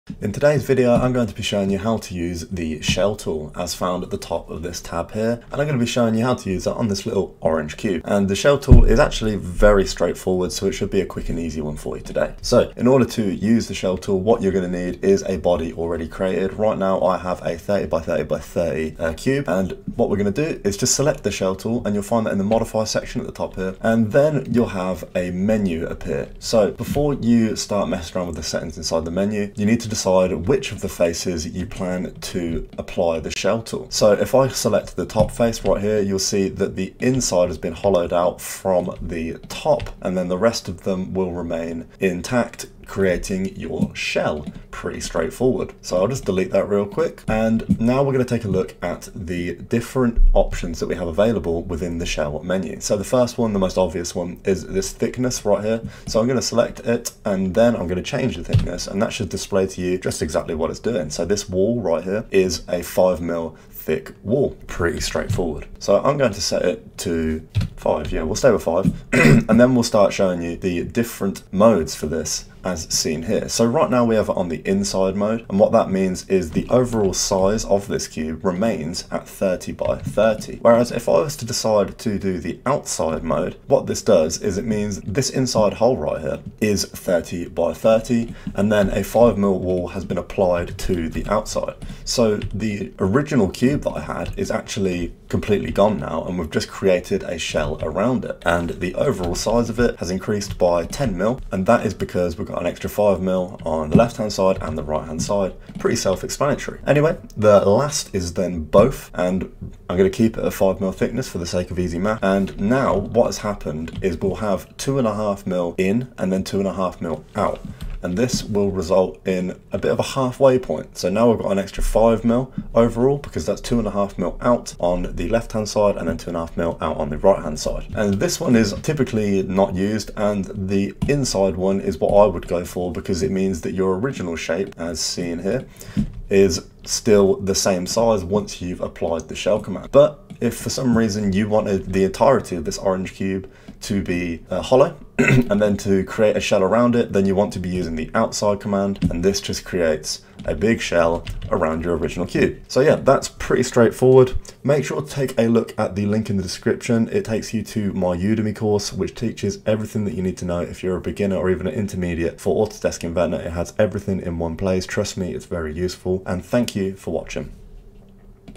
How to use the Shell Tool - Autodesk Inventor
How to use the different modes within the Shell tool found in Autodesk Inventor